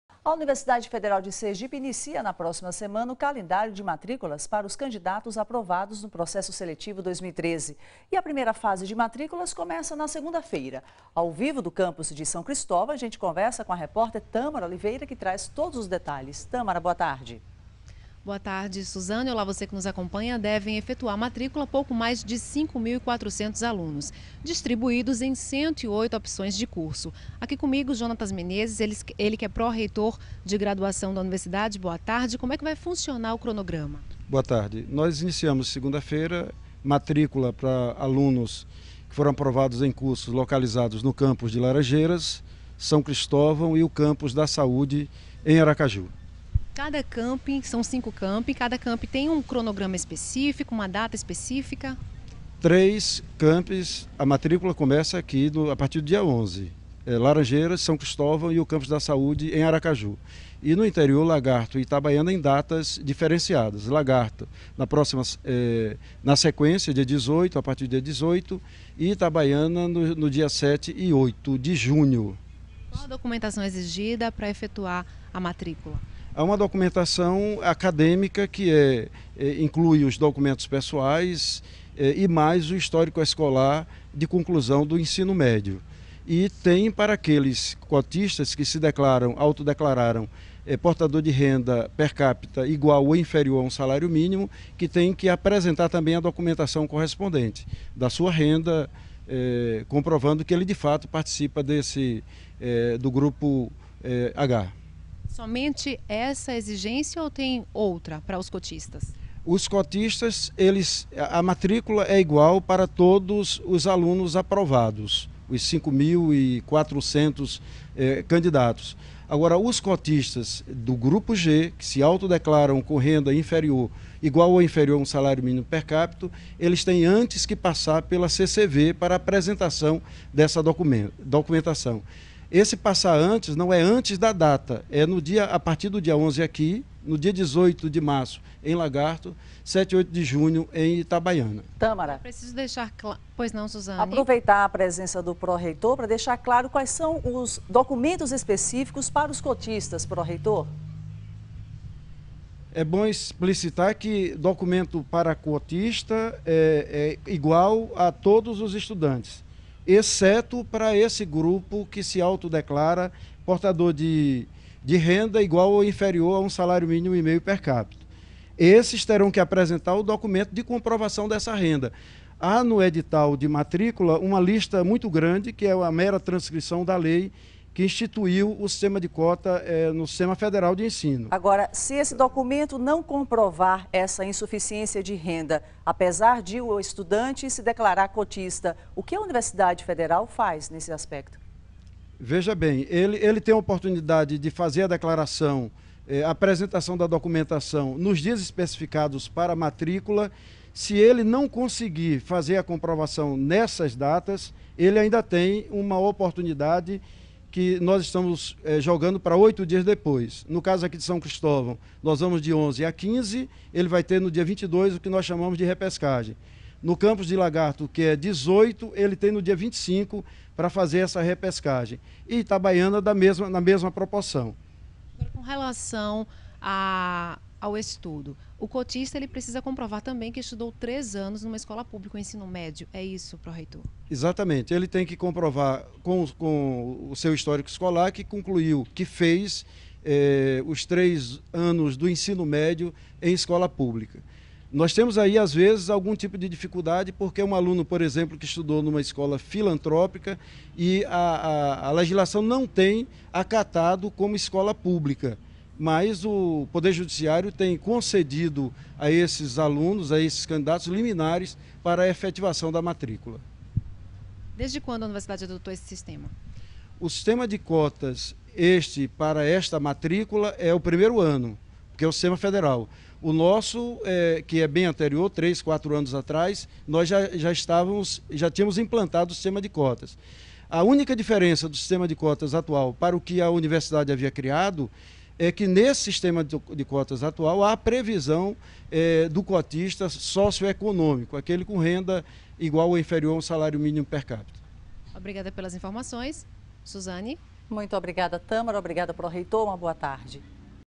Acervo | Mais de 5,4 mil alunos aprovados vestibular da UFS devem se matricular
Data: 5 mar 2013.